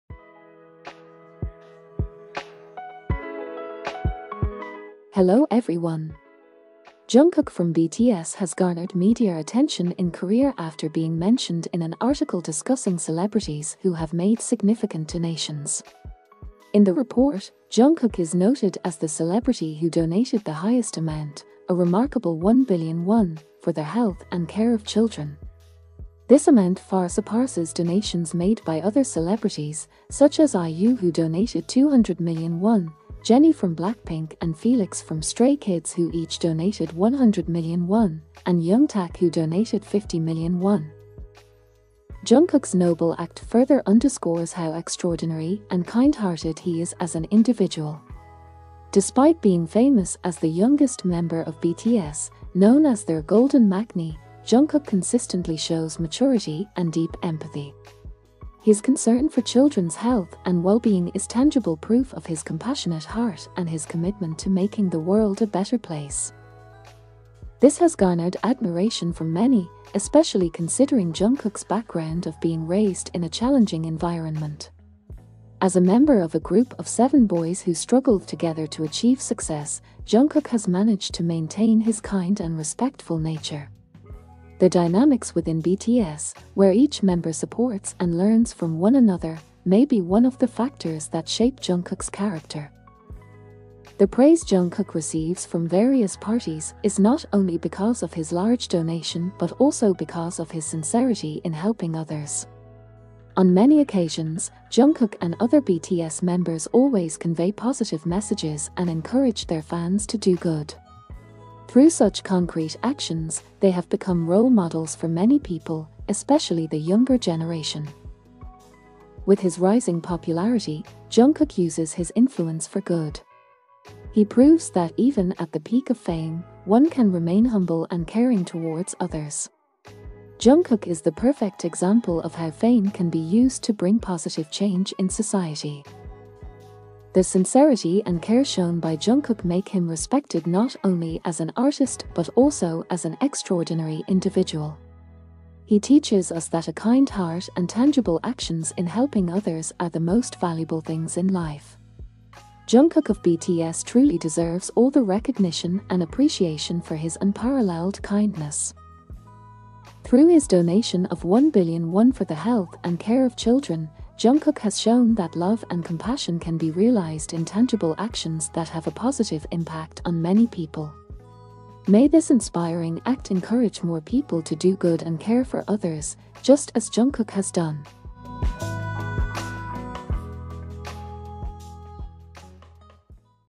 This is unexpectedly what happened to Jungkook
Bts news today! Jungkook BTS: 1 Billion Won Donation for Children, Proof of the Golden Maknae's Heart of Gold.

Jungkook from BTS has garnered media attention in Korea after being mentioned in an article discussing celebrities who have made significant donations. In the report, Jungkook is noted as the celebrity who donated the highest amount, a remarkable 1 billion won, for the health and care of children. This amount far surpasses donations made by other celebrities, such as IU who donated 200 million won, Jennie from BLACKPINK and Felix from Stray Kids who each donated 100 million won, and Young Tak who donated 50 million won.